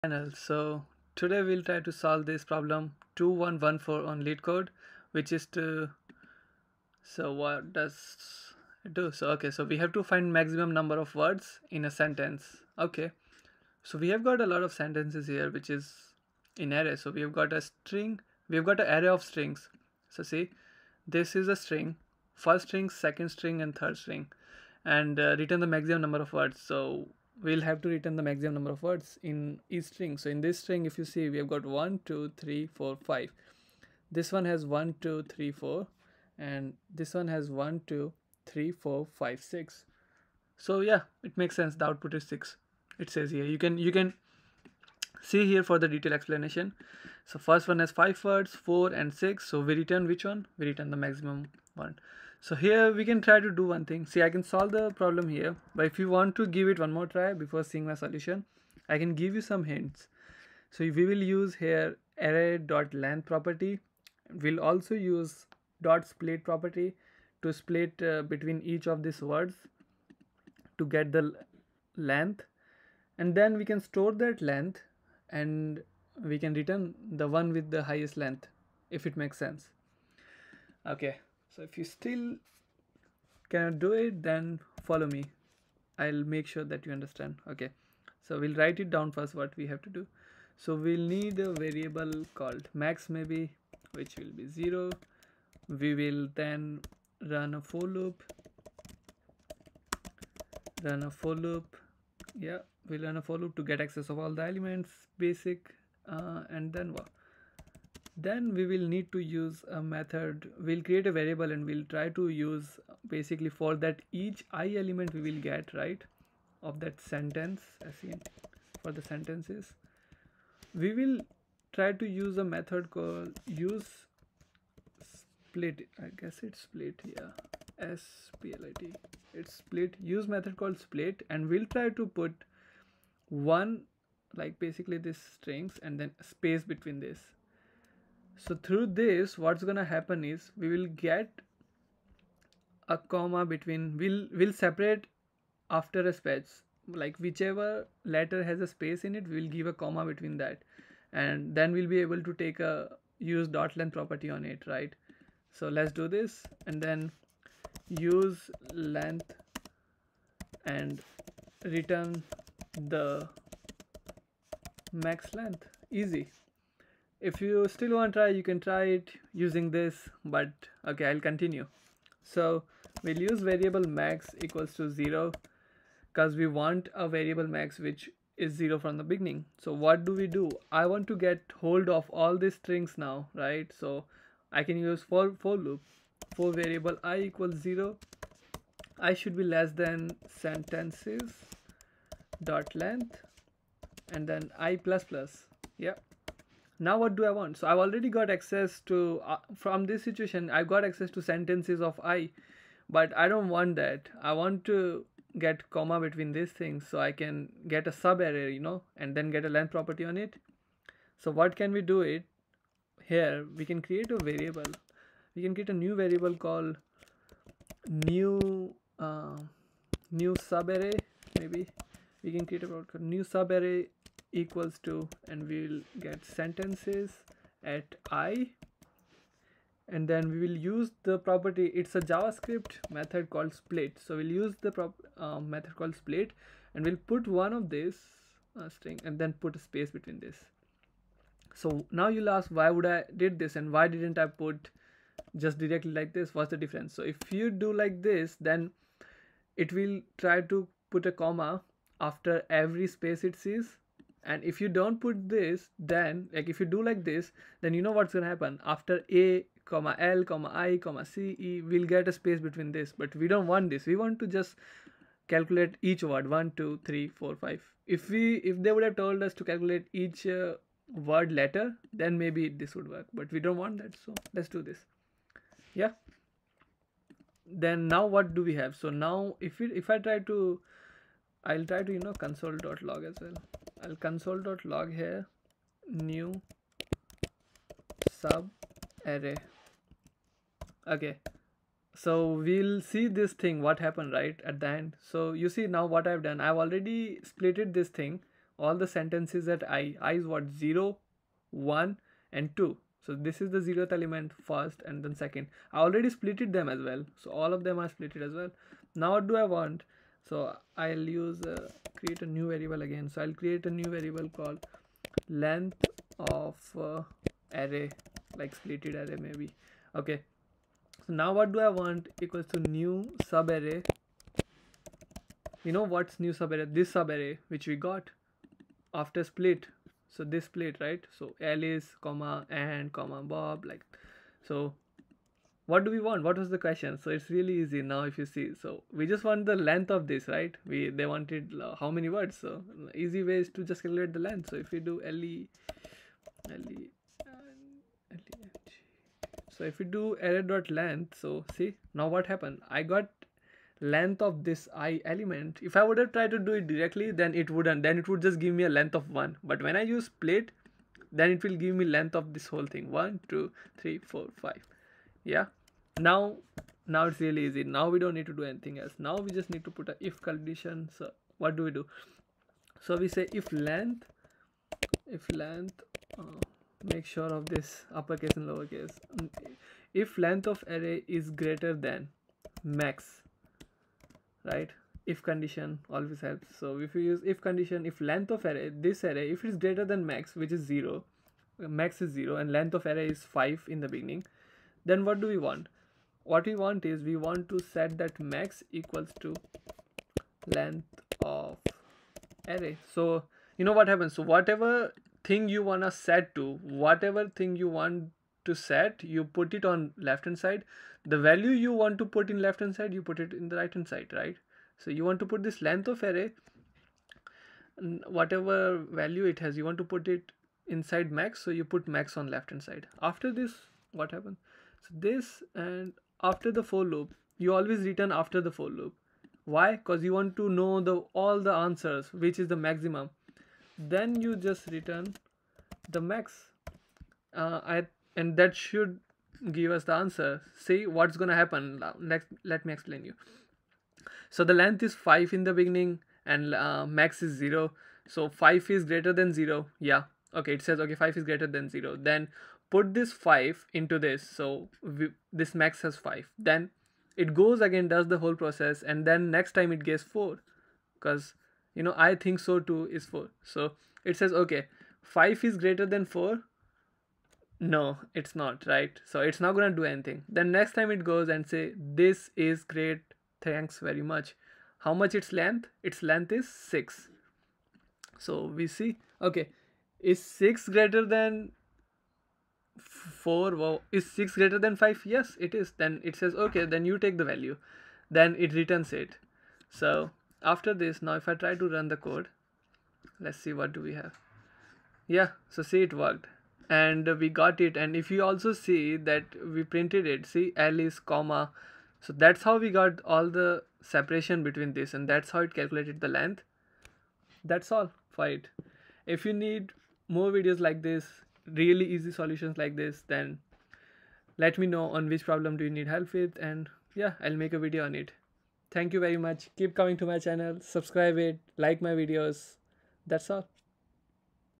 LEETCODE SOLUTION 2114 | Maximum Number of Words Found | Leetcode for BEGINNERS | EASY | Javascript
LEETCODE SOLUTION no 2114

Today i will be solving LEETCODE SOLUTION 2114, Maximum Number of Words Found in Sentences. It's super easy for beginners. I used javascript to solve this.  I hope this video will help you. If you have any question, you can comment and i will reply.